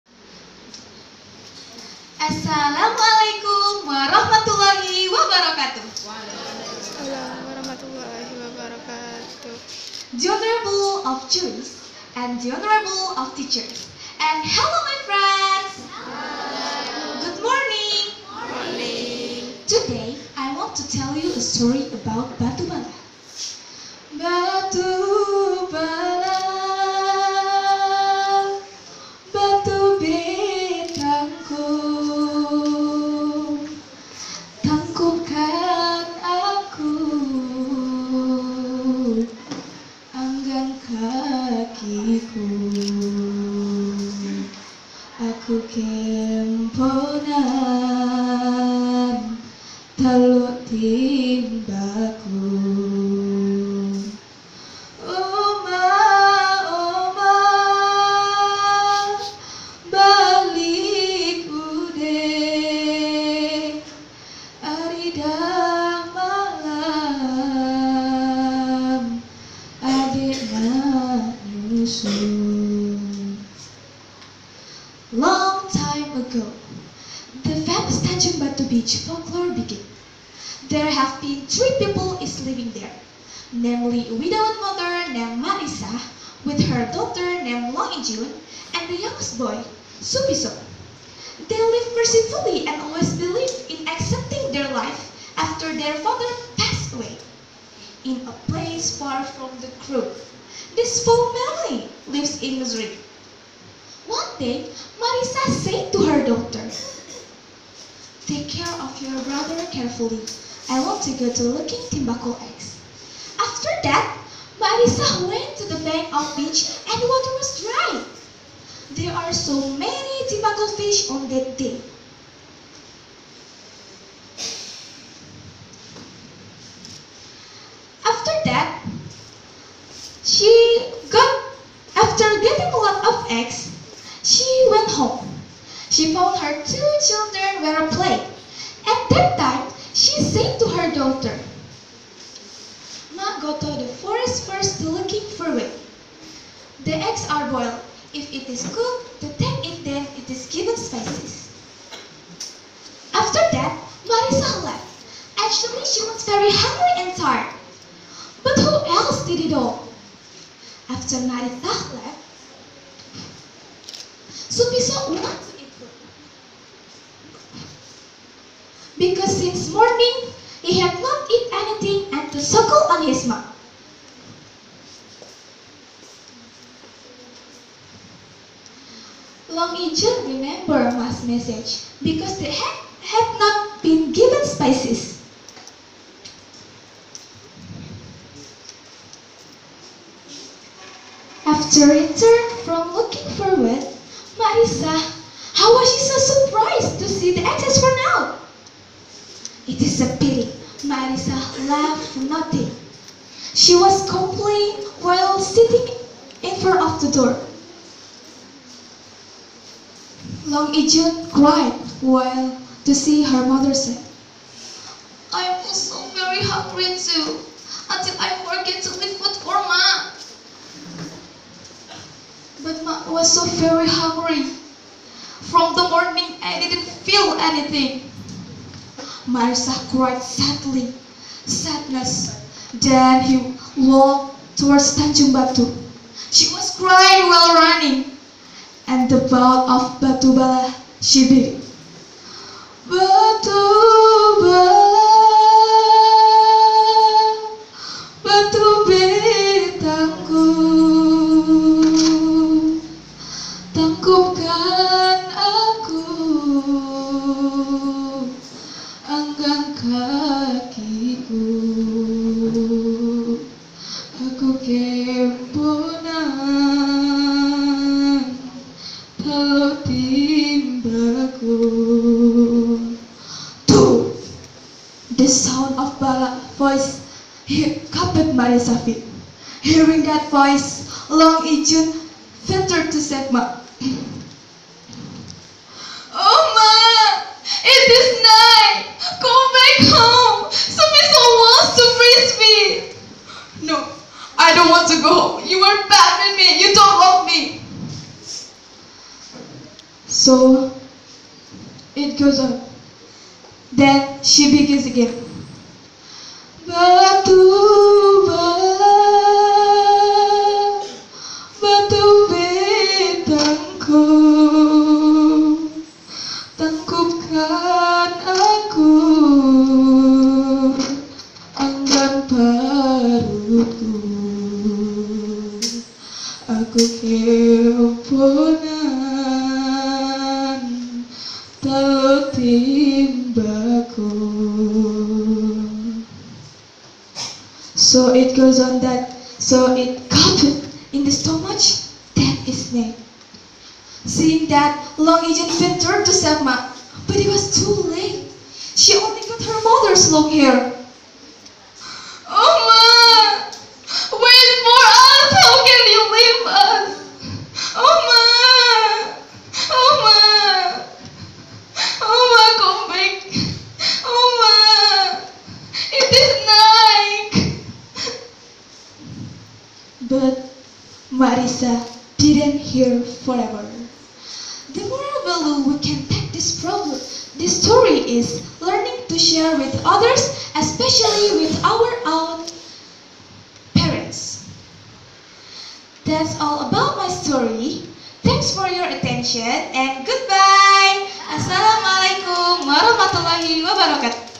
Story Telling Batu Belah Batu Betangkup by Alya
Cerita Rakyat kalimantan barat di ceritaksn oleh siswa MTsN 1 Singkawang dalam bahasa inggris